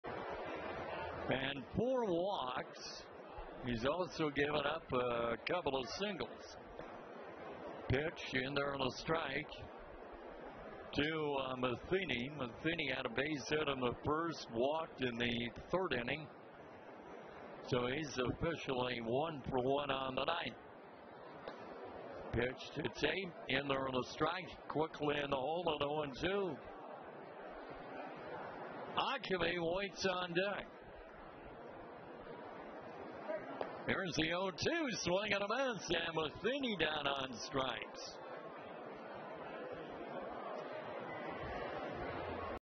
Intimidators' Martinez fans ninth batter
8/18/16: Kannapolis' Luis Martinez strikes out his ninth batter as the Intimidators fall to Greenville, 2-0, on Thursday